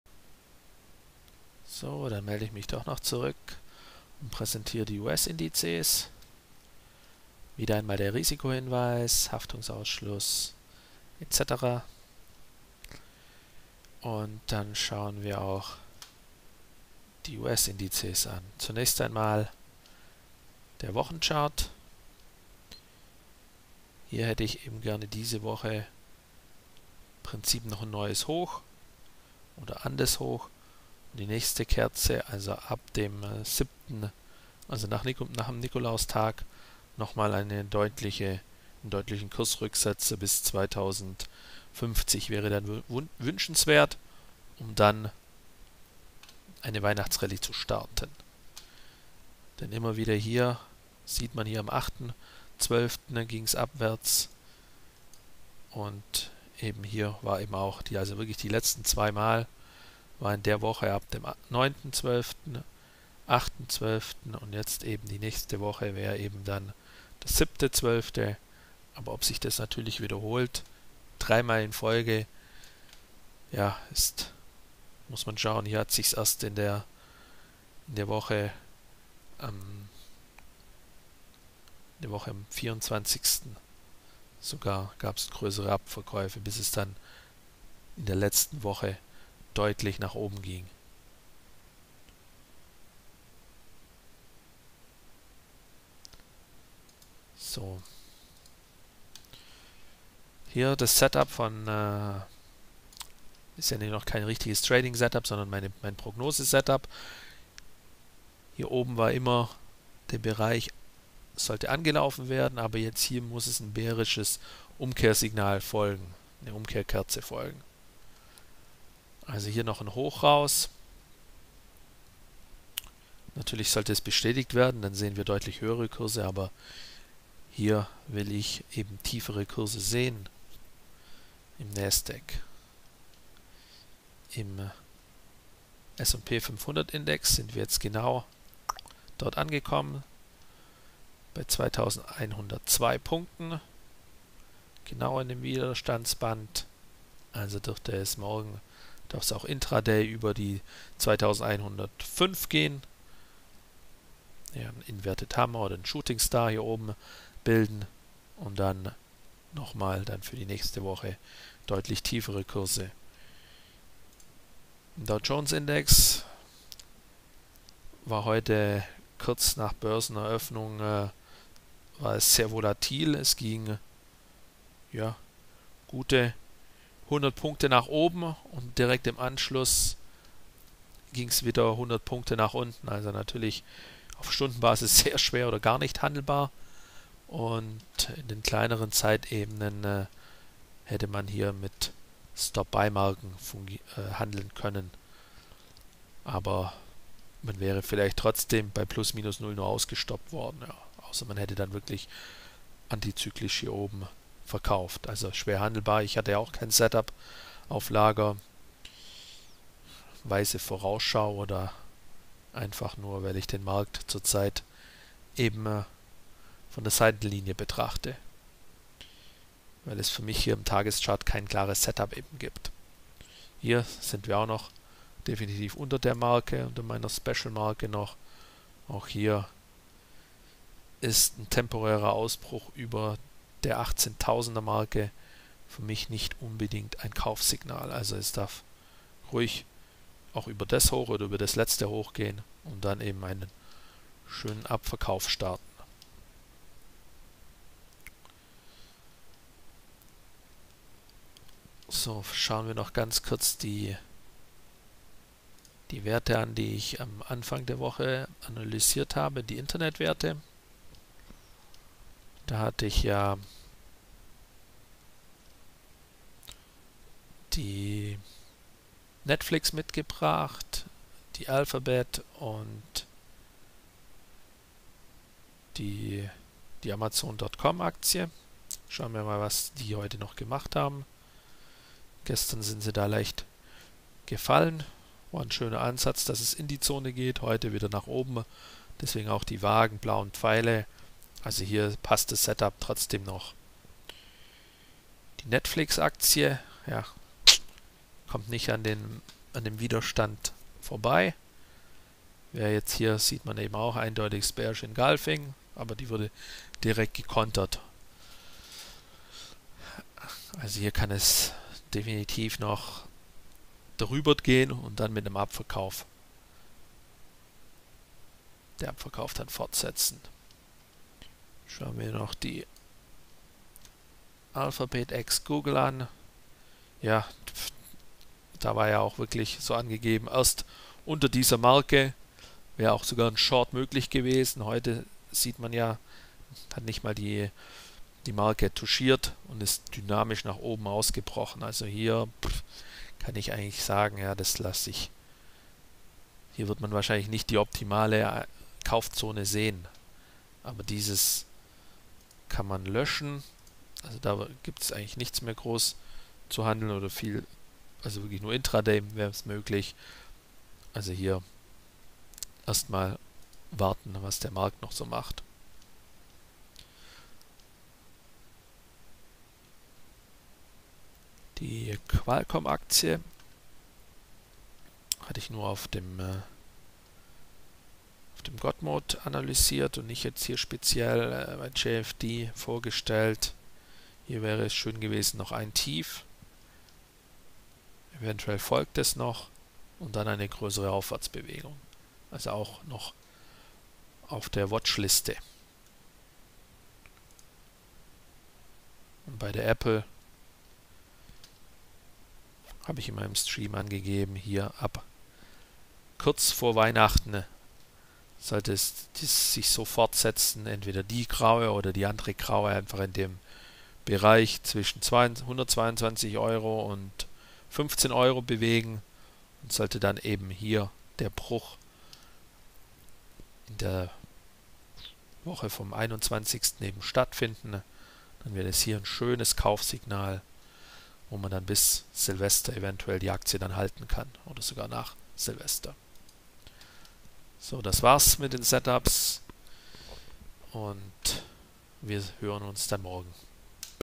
US-Indizes - Nach(t)-Beschau
Chartbesprechungen: Nasdaq100. Dow und S&P500 + Amazon.com Alphabet und Netflix.